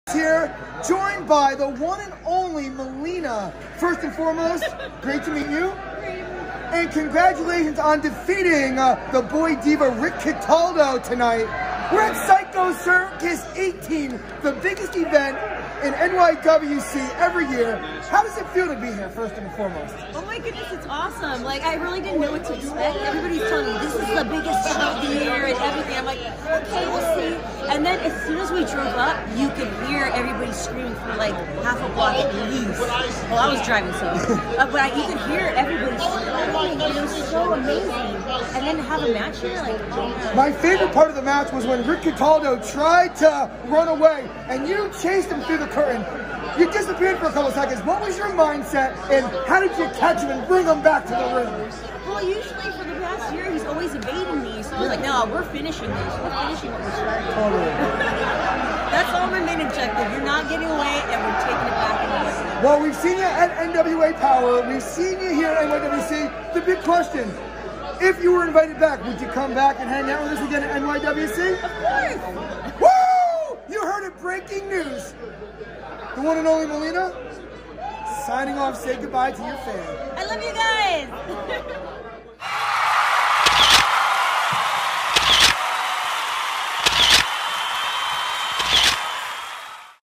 NYWC (2/22/2020): Melina Talks About Her Psycho Circus 18 Experience
NYWC: Psycho Circus 18
NYWC Sportatorium: Deer Park, NY
***Announcer/Backstage Correspondent David Adams Talks to Melina About Her NYWC PSYCHO CIRCUS 18 Experience***